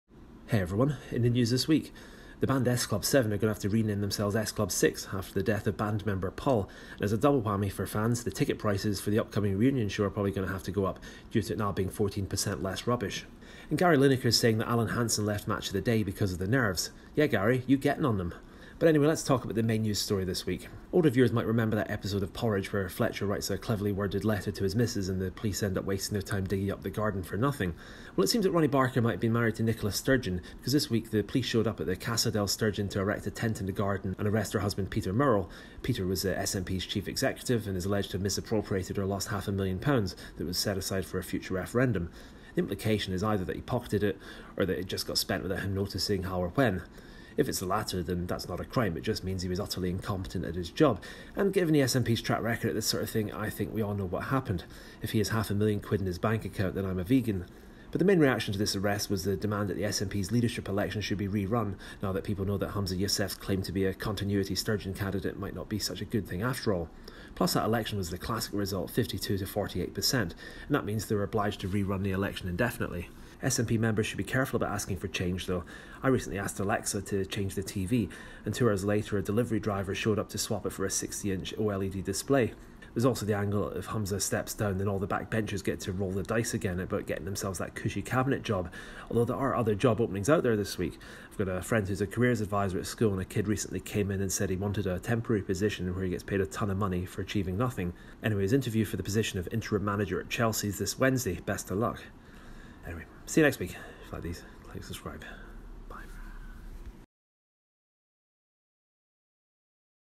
2023 Apr 9 - Peter Murrell Arrested
News summary for a week in which Nicola Sturgeon's husband was arrested